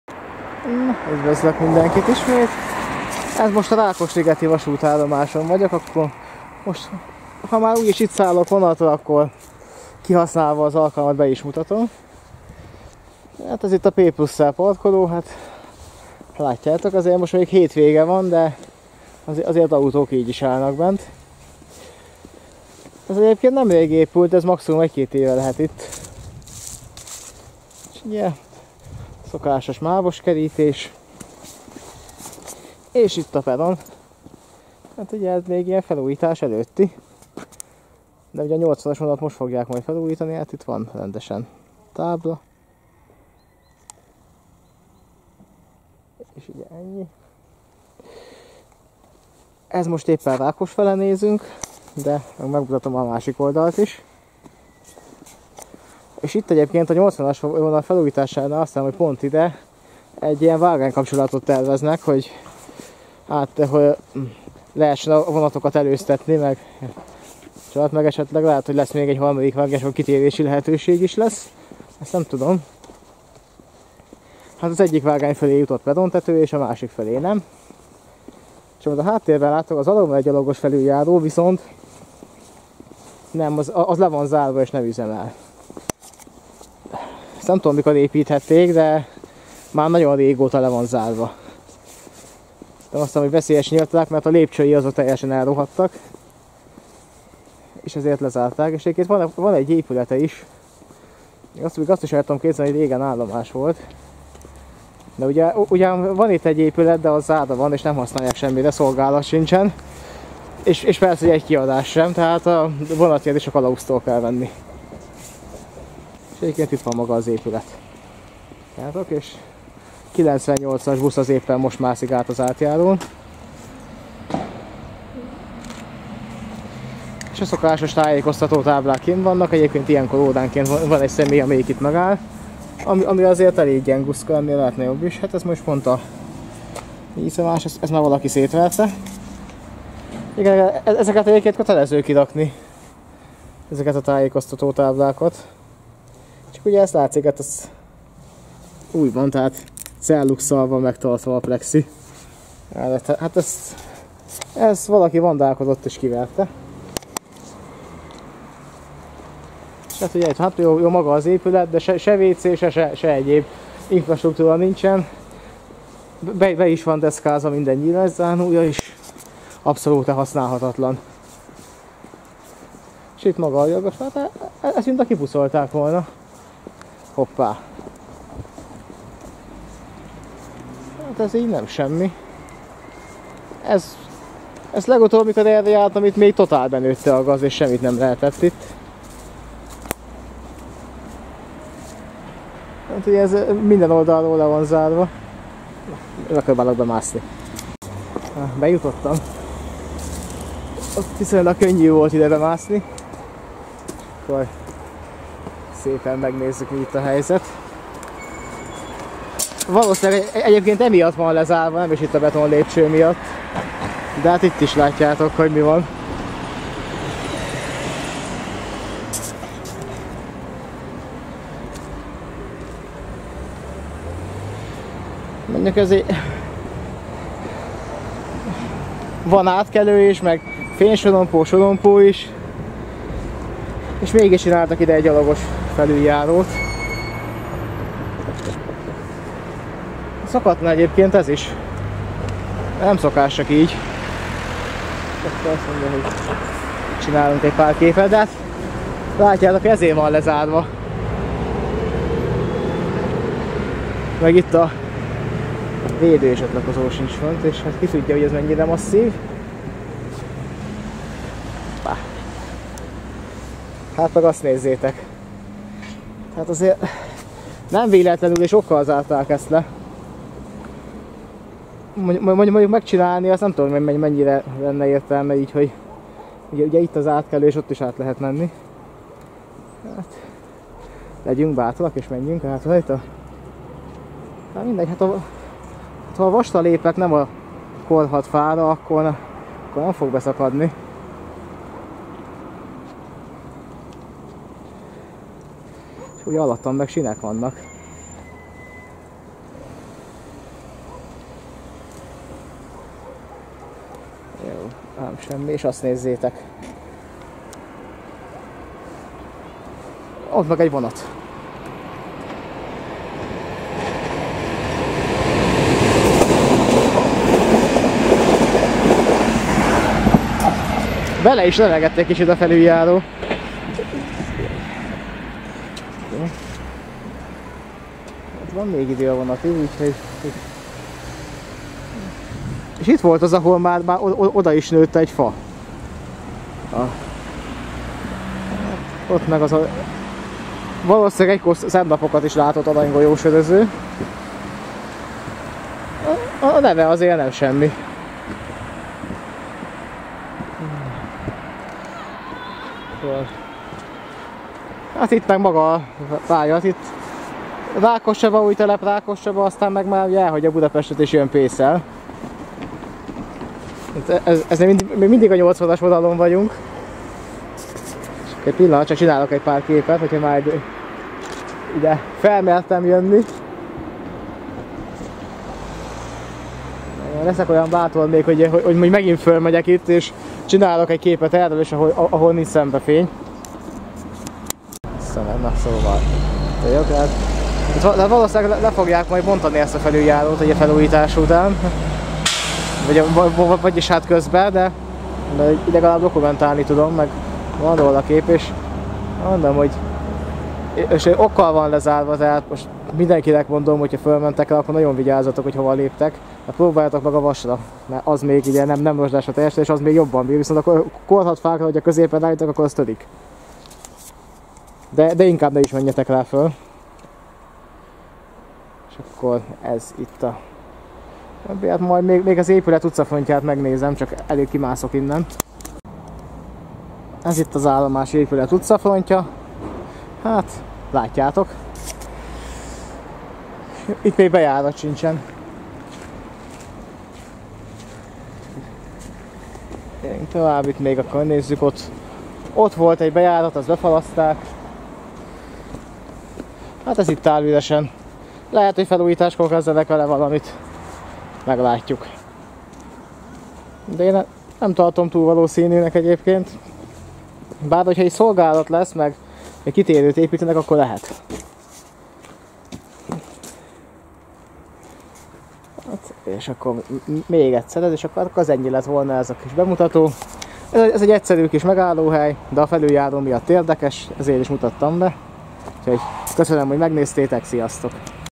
2018.01.27 Rákosliget megállóhely bemutatása
Rákosligeten jártam, kihasználva, hogy van még idő a vonatig videóztam. A felüljárót szinte biztosan bontani fogják a felújítás után, ami idén kezdődik.